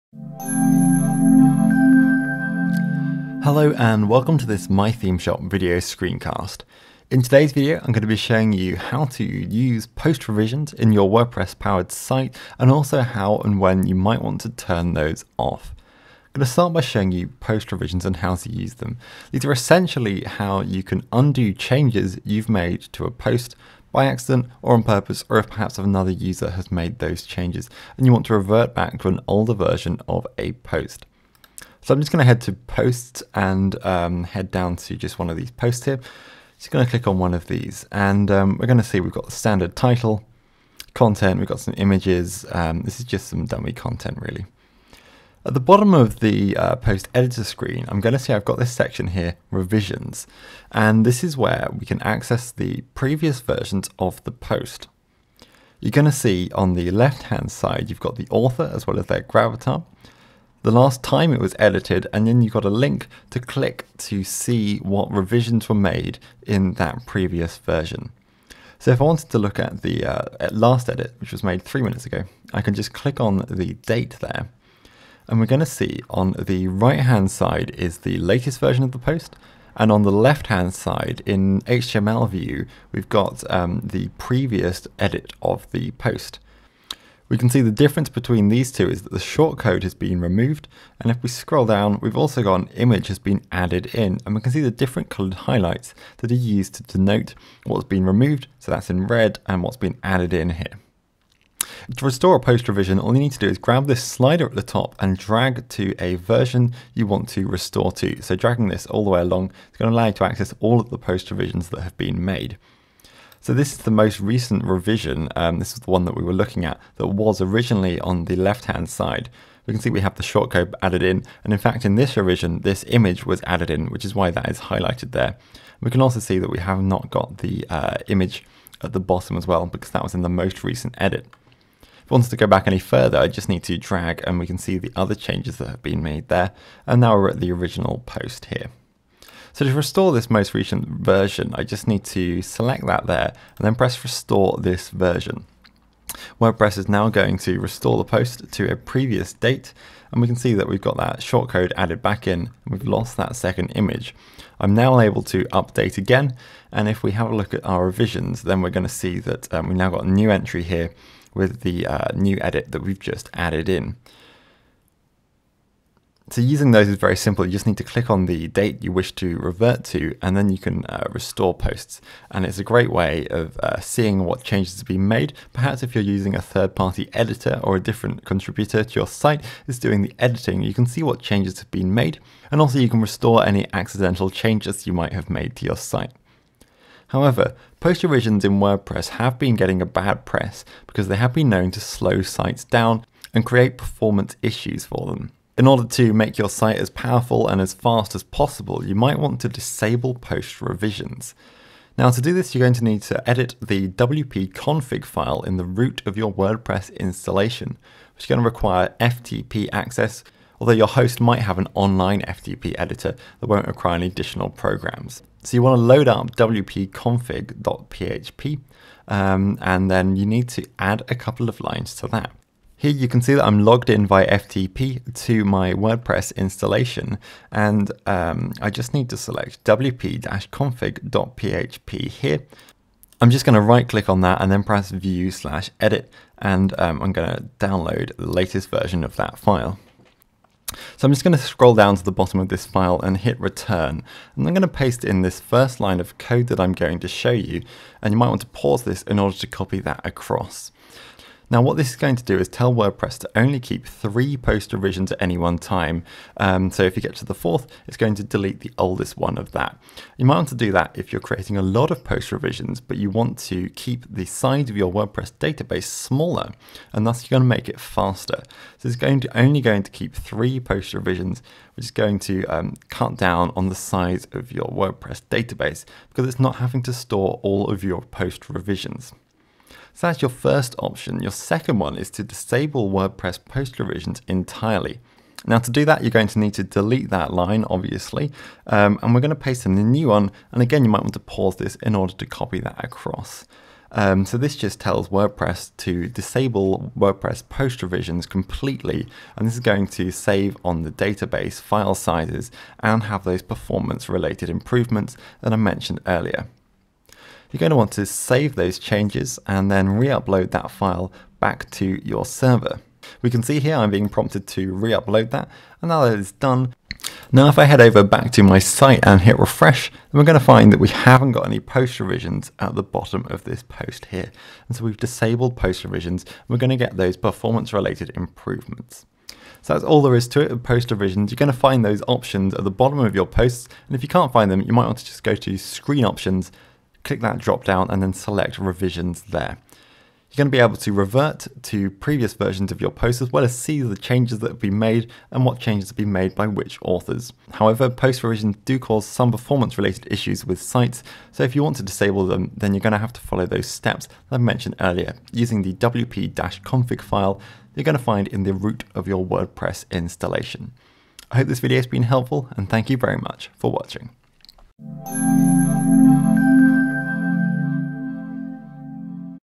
WordPress 201 - Post Revisions
Understand WordPress Post Revisions in depth. Watch the above video tutorial to configure the WordPress posts.